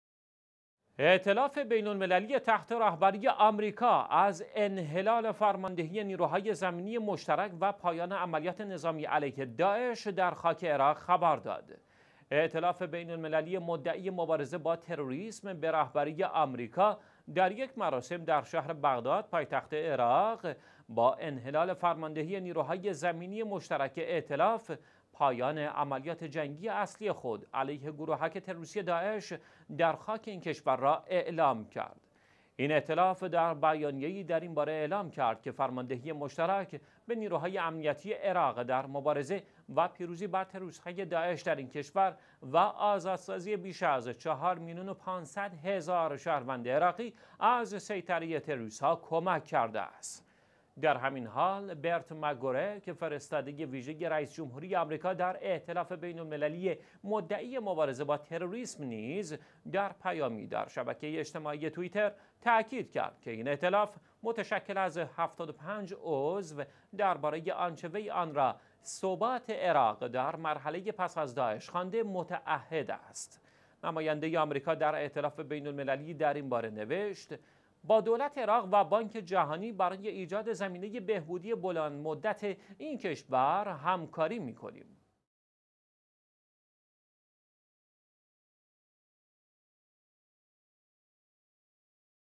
اعلام پايان عمليات ائتلاف بين‌المللي عليه داعش در عراق
ائتلاف بین_المللی تحت رهبری آمریکا از انحلال فرماندهی نیروهای زمینی مشترک و پایان عملیات نظامی علیه داعش در خاک عراق خبر داد.
ائتلاف بین المللی مدعی مبارزه با تروریسم به رهبری آمریکا، در یک مراسم در شهر بغداد، پایتخت عراق که شاهد انحلال فرماندهی نیروهای زمینی مشترک ائتلاف بود، پایان عملیات جنگی اصلی خود علیه گروهک تروریستی داعش در خاک این کشور را اعلام کرد.
این ائتلاف، در بیانیه ای در این باره اعلام کرد که فرماندهی مشترک به نیروهای امنیتی عراق در شکست تروریست های داعش در این کشور و آزادسازی بیش از چهار میلیون و ۵۰۰ هزار شهروند عراقی تحت سیطره آنها کمک کرده است.
در همین حال، «برت مک گورک»، فرستاده ویژه رئیس_جمهور ی آمریکا به ائتلاف بین المللی مدعی مبارزه با تروریسم نیز در پیامی در شبکه اجتماعی توییتر تاکید کرد که این ائتلاف متشکل از ۷۵ عضو نسبت به آنچه وی آن را ثبات عراق در مرحله پس از داعش خوانده؛ متعهد است.
نماینده آمریکا در ائتلاف بین المللی در این باره نوشت: با دولت عراق و بانک جهانی برای ایجاد زمینه بهبودی بلندمدت این کشور کار می کنیم.